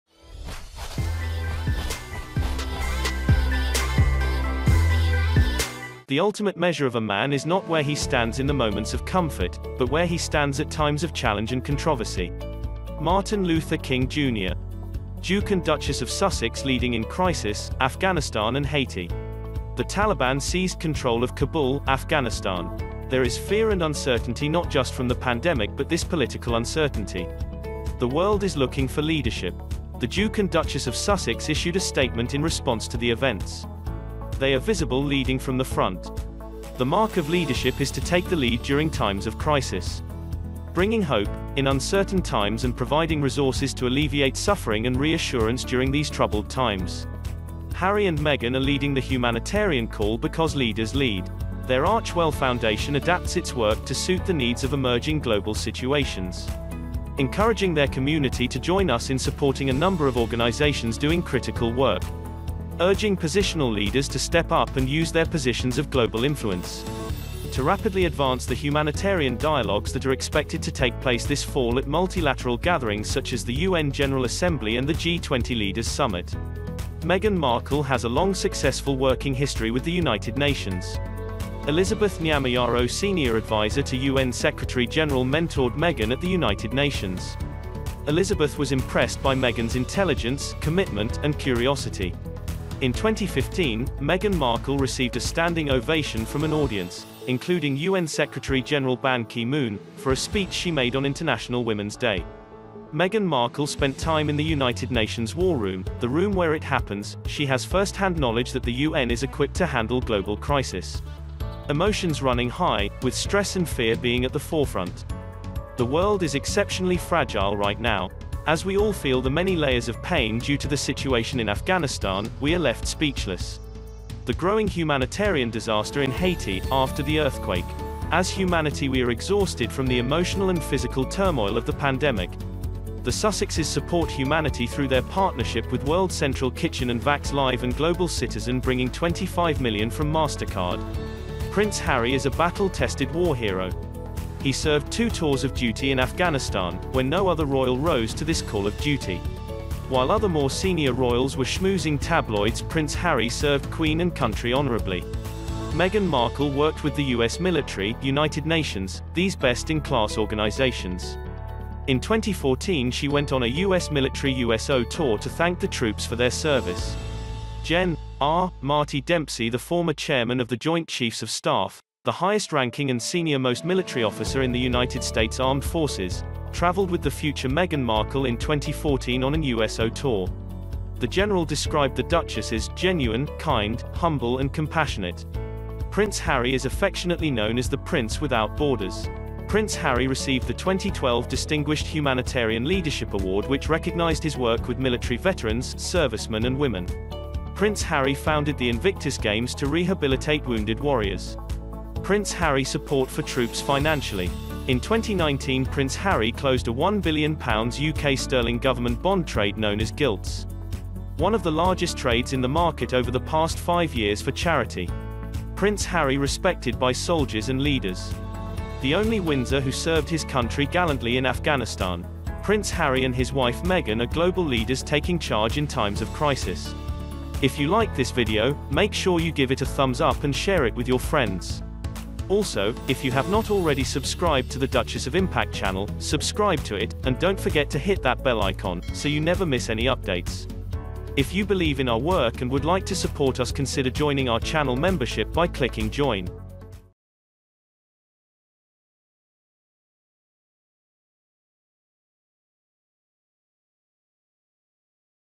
Duke & Duchess of Sussex Leading In Times of Crisis| Afghanistan & Haiti
There is fear and uncertainty not just from the pandemic but this political uncertainty. The world is looking for leadership. The Duke and Duchess of Sussex issued a statement in response to the events. They are visible leading from the front.
“The ultimate measure of a man is not where he stands in the moments of comfort, but where he stands at times of challenge and controversy.” Martin Luther King, Jr.